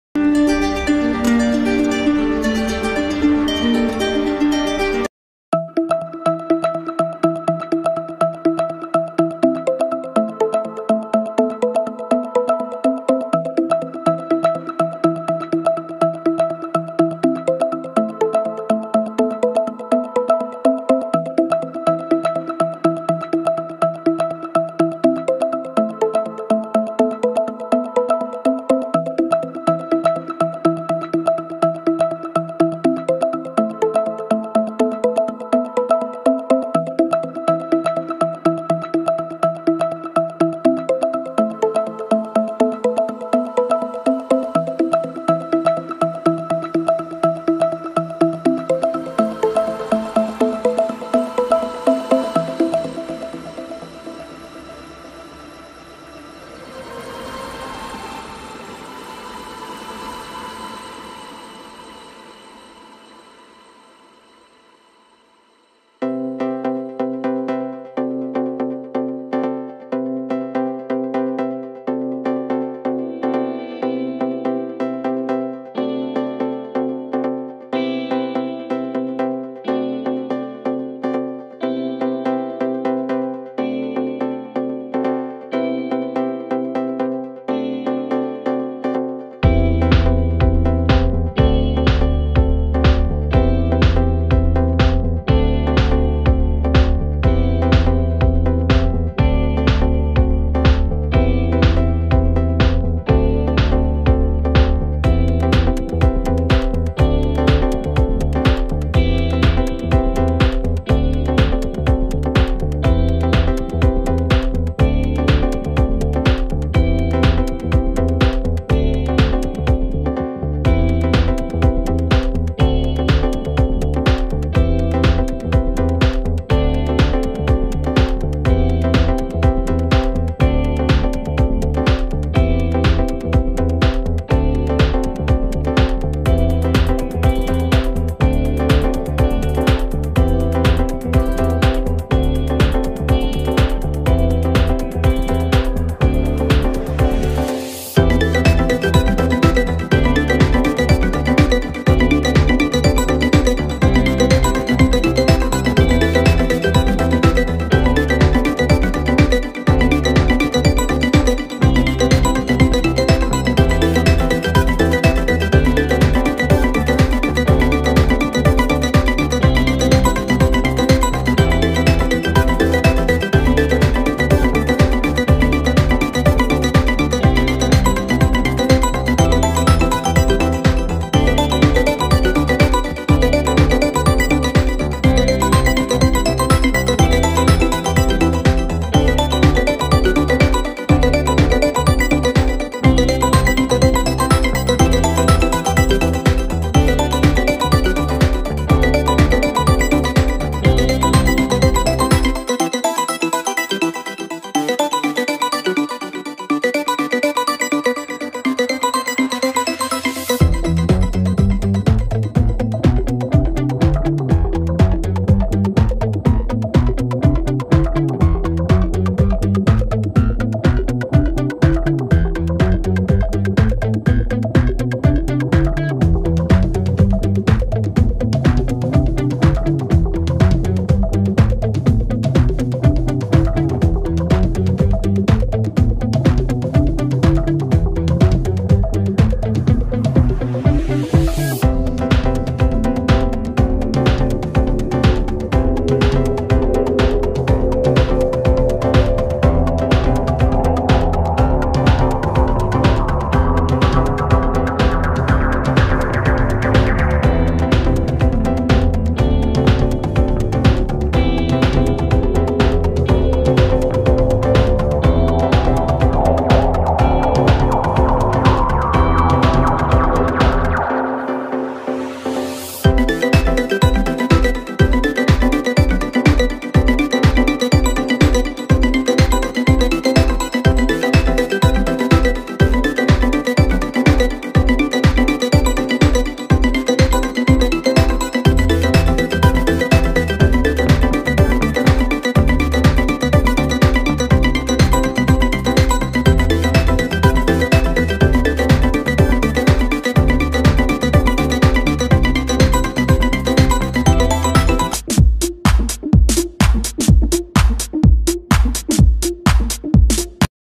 Old textile mill in Nexus Bd
It is old textile mills in nexus bd. This is our attempt to remind you of the past. We will always try to give you some good gifts.